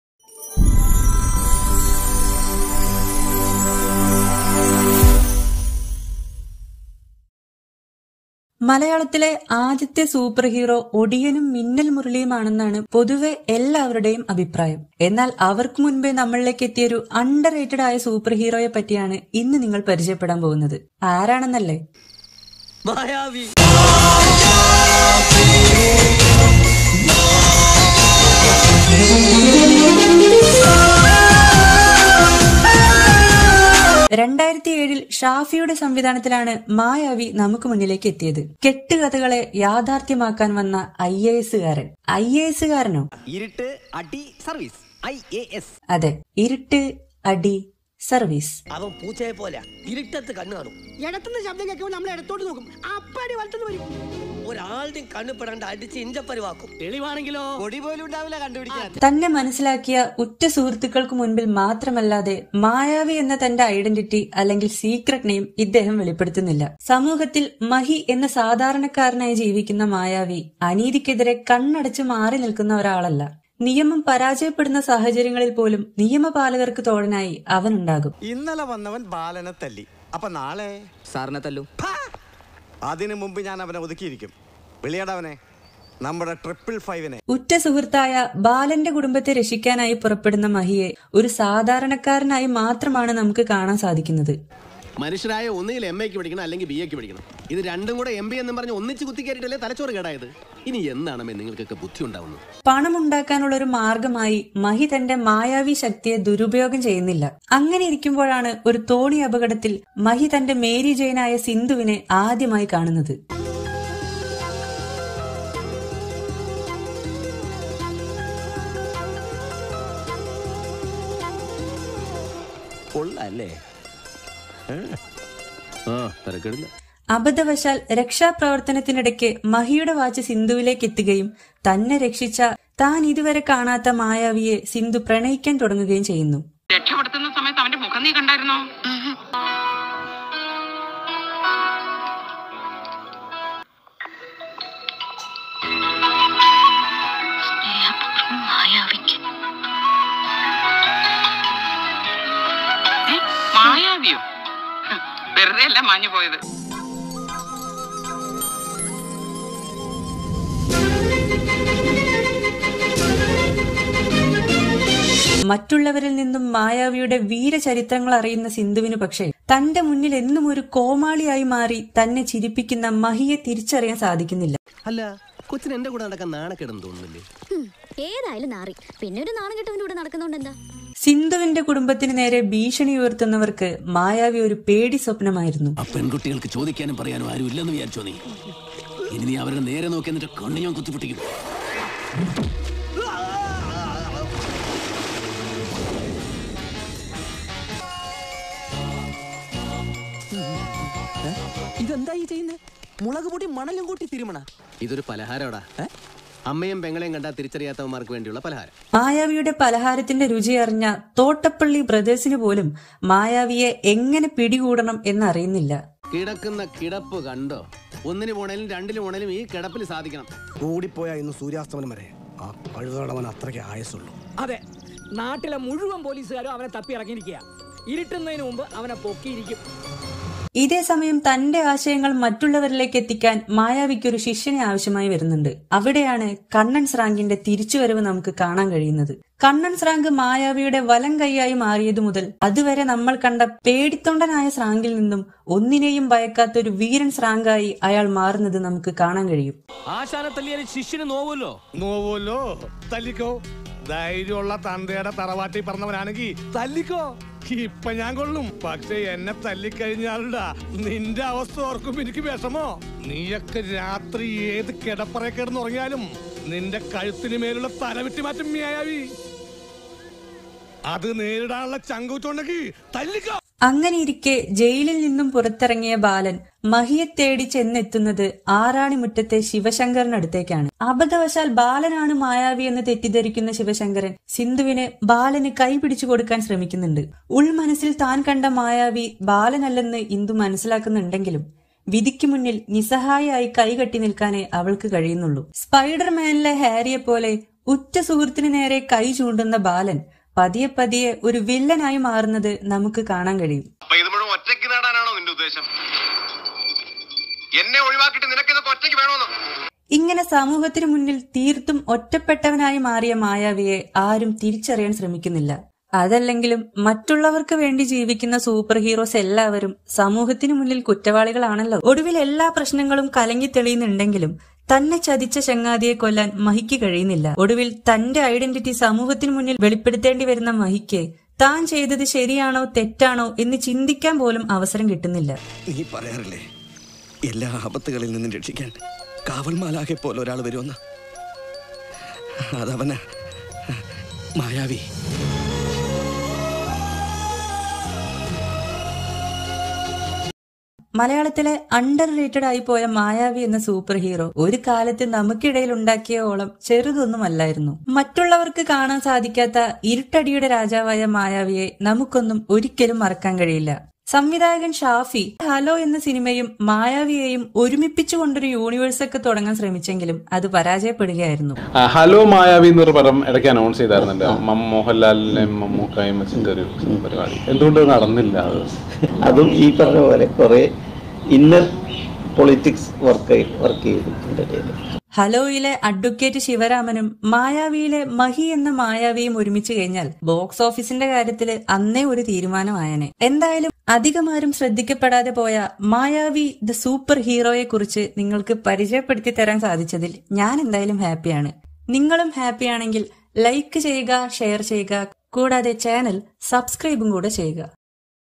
Daredevil of Mollywood|Malayalam|My Turn
This video is about the underrated superhero in malayalam film industry.